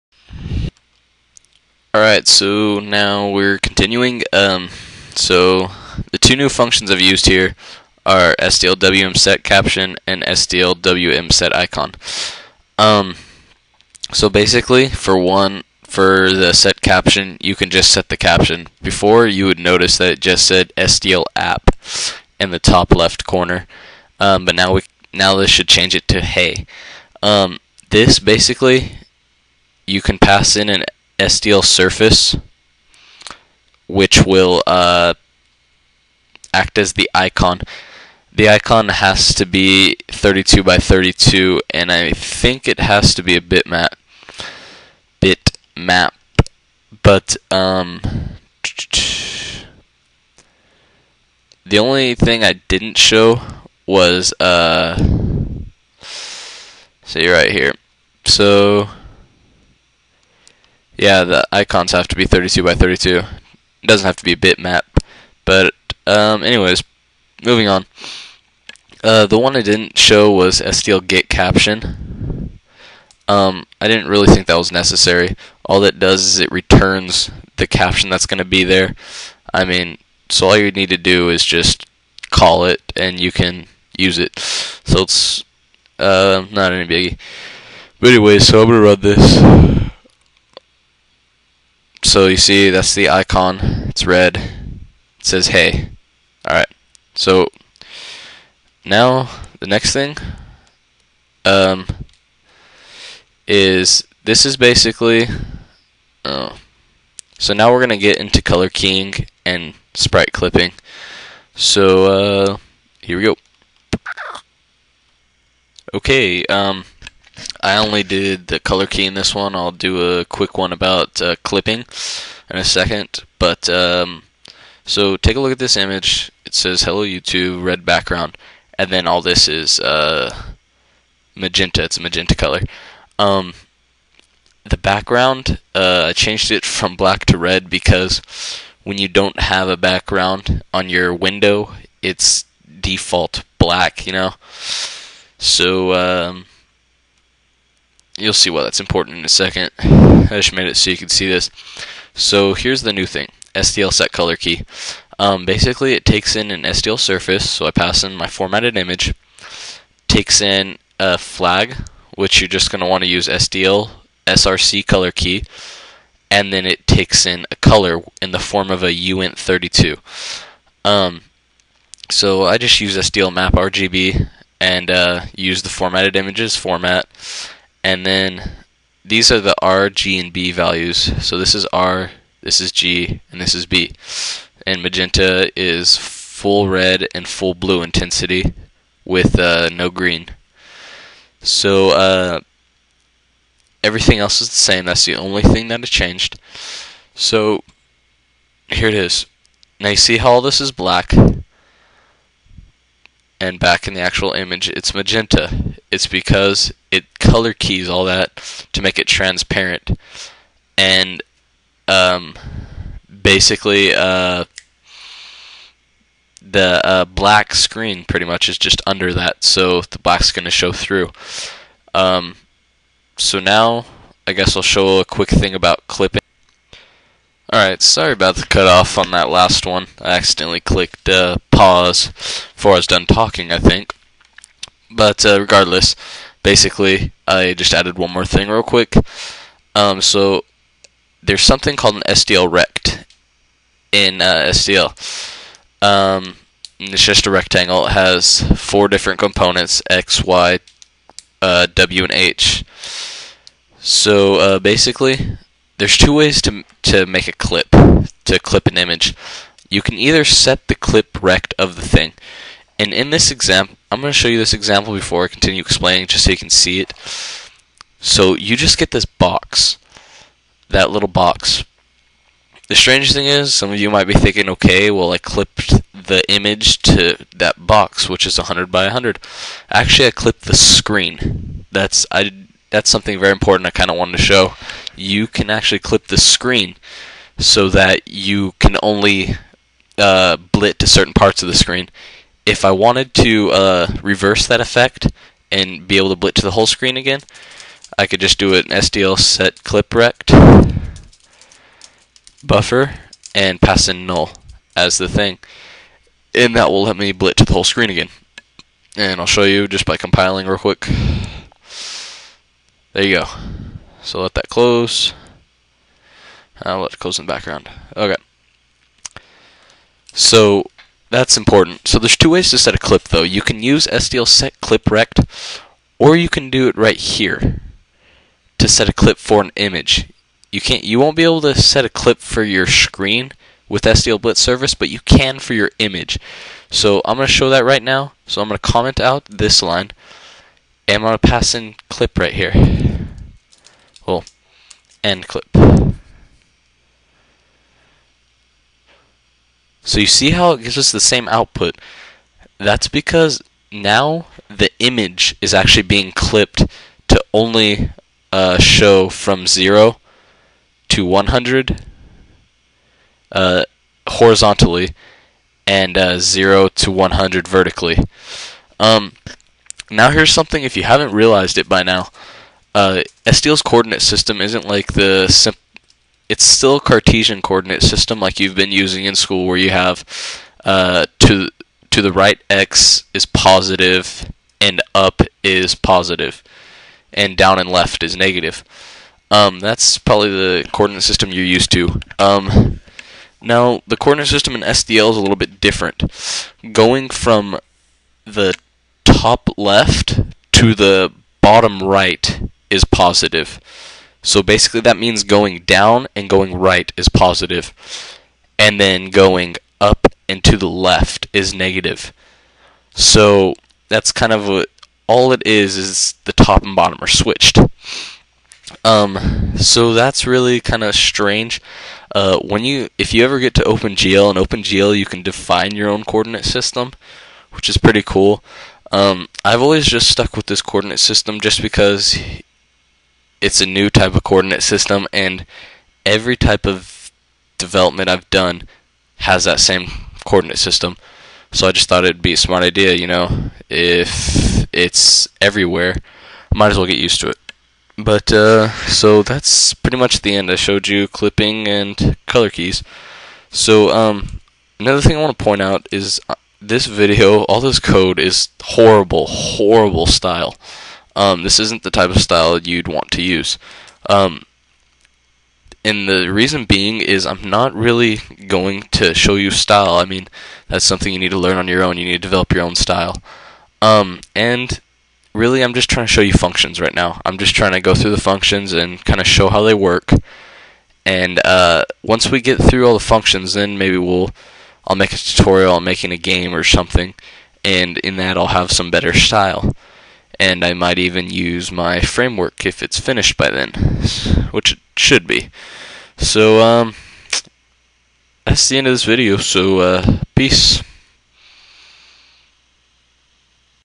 SDL Tutorial 2 (Part 2)
Second Part of my SDL tutorial dealing with images and window managing.

This video will show you how to load, format, and color key images along with how to set up SDL_image.